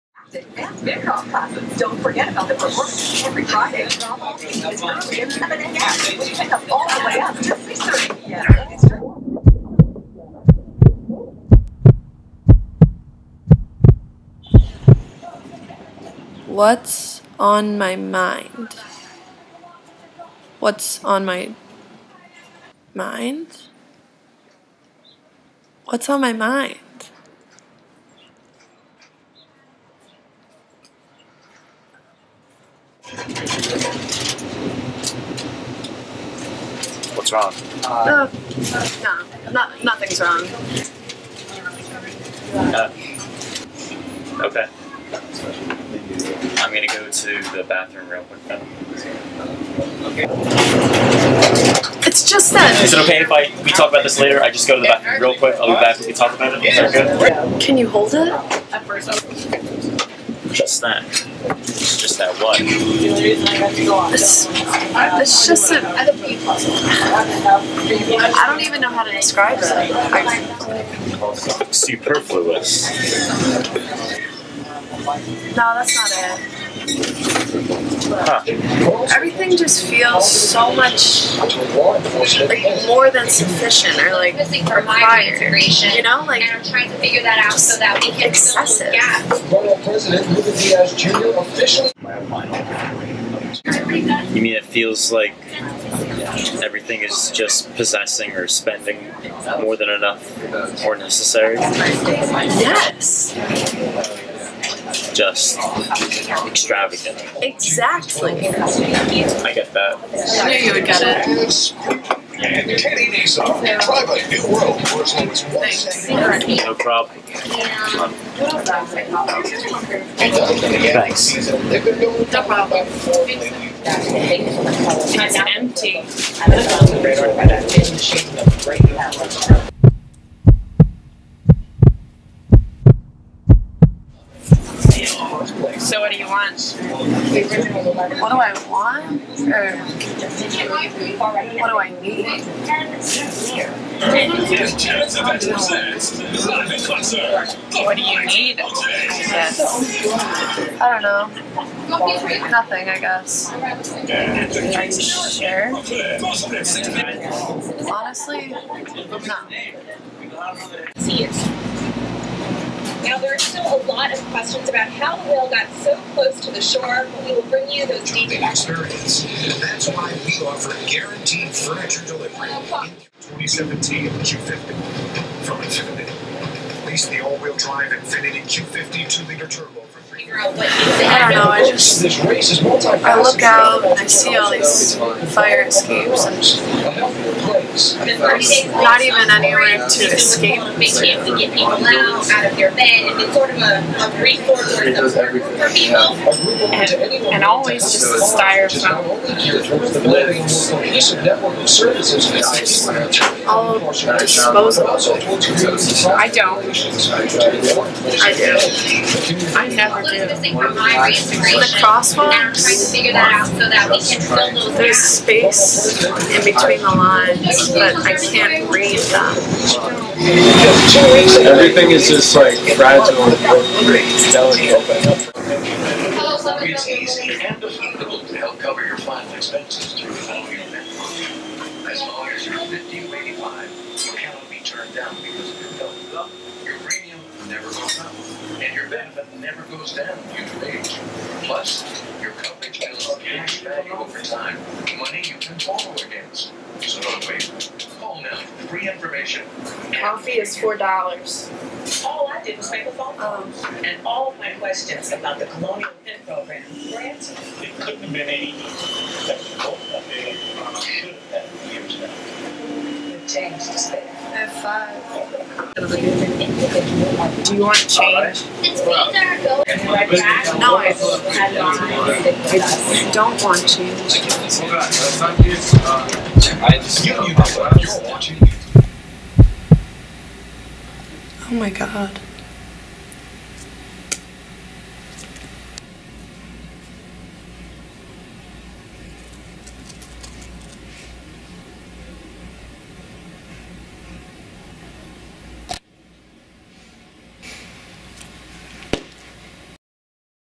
What's On Your Mind?
A millennial grapples with the daunting task of summarizing one's inner cognitive mechanisms, dreams, and fears within the platform of a Facebook status.

Created by Emma Carey's Crew at Fordham University in 2017 as part of Campus MovieFest, the world’s largest student film festival.

Cast & Crew :
Emma Carey - Captain, Writer, Actor, Director
with
Erin Kiernan - Actor
Mario Nicastro - Actor, Cinematographer

Make sure to check out this video and thousands more on the Campus MovieFest